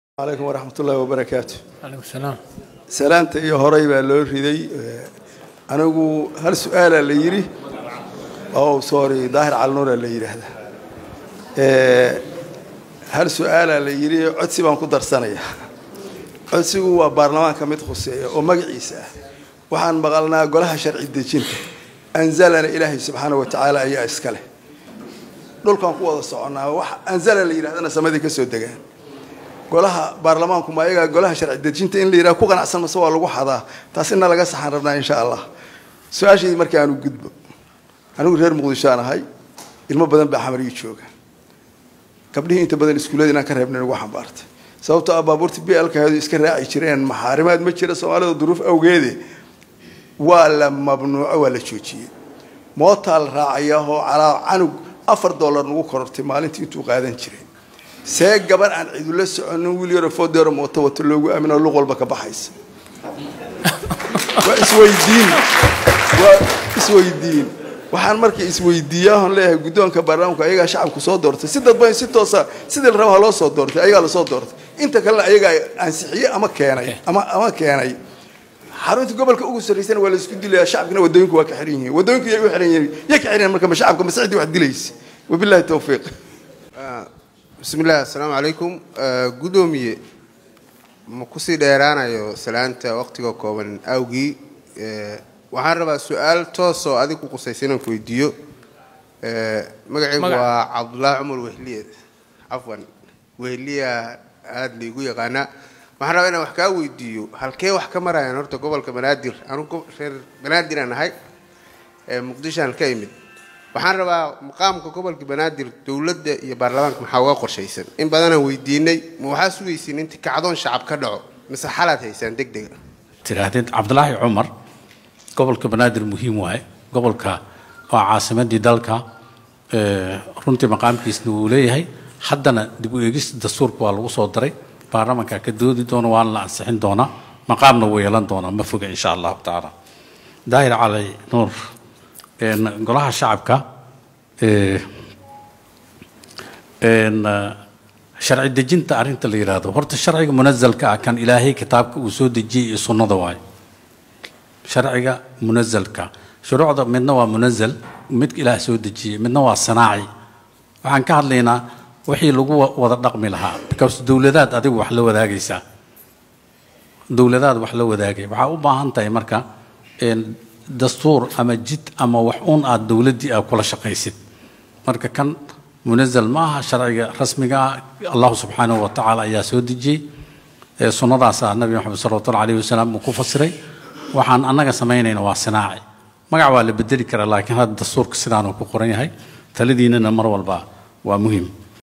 MAXAA KALA HEYSTA GABDHAHA MUQDISHO IYO MOOTA BAJAAJTAJAALIYADDA SOOMAALIYEE
Su'aalo la weydiiyay Guddoomiyaha Baalamaanka Soomaaliya Maxamed Mursal isagoo ku sugan magaalada Cape Town ee dalka South Africa.